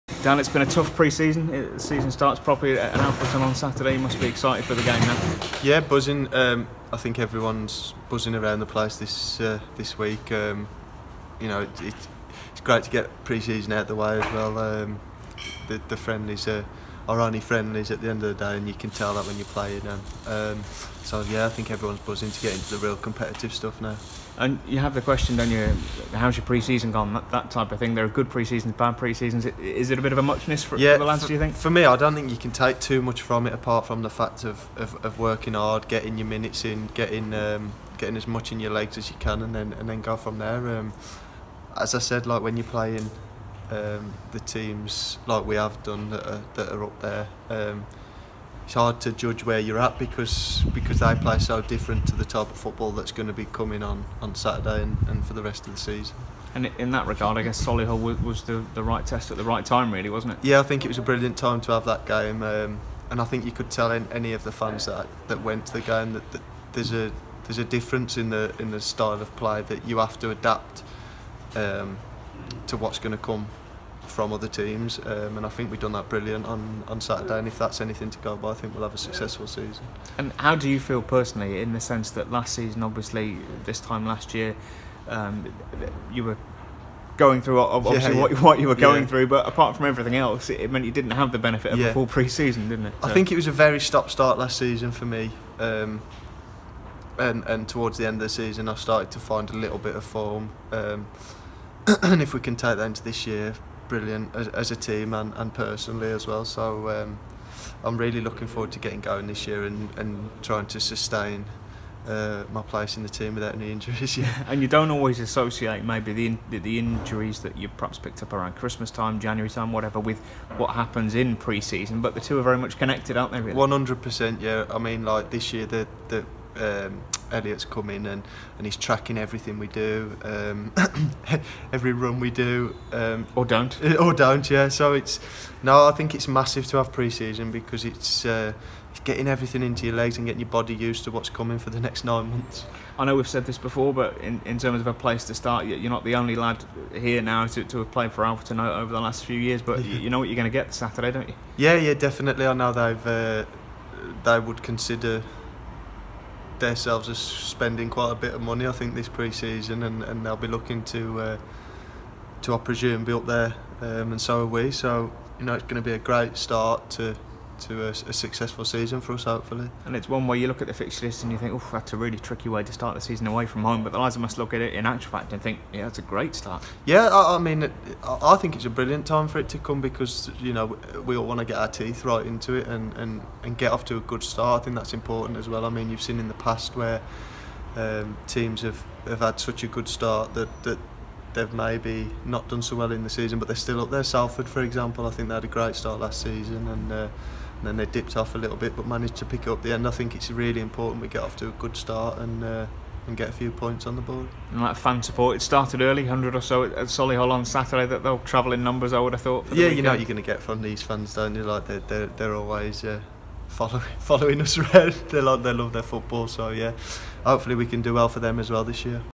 Dan Bradley pre-match: Alfreton Town v Harriers 04/08/18
With the new season upon us, Dan Bradley looks to the Alfreton Town game